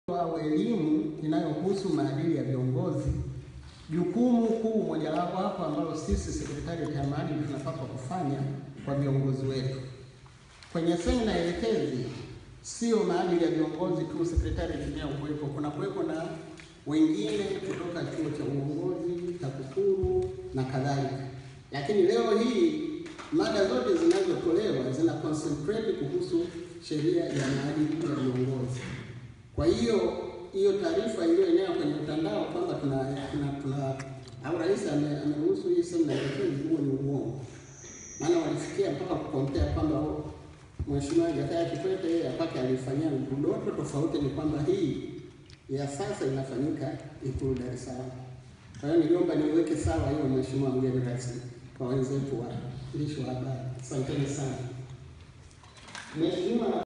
Semina Elekezi kwa Mawaziri - Feb 25, 2016
Tume ya Maadili ikitoa ufafanuzi wa sababu ya kuandaa Semina hii na lengo lake